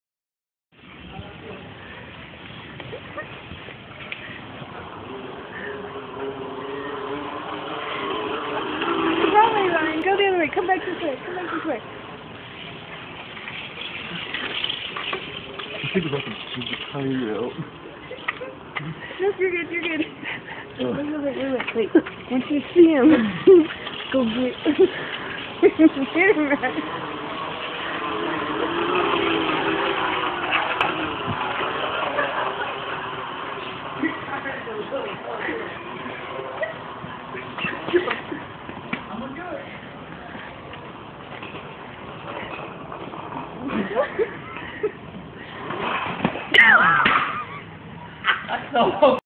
Josh and Ryan Racing in Wal Mart
Ryan and Josh racing around wal mart on toddler bikes! Then Ryan tries to do a wheelie!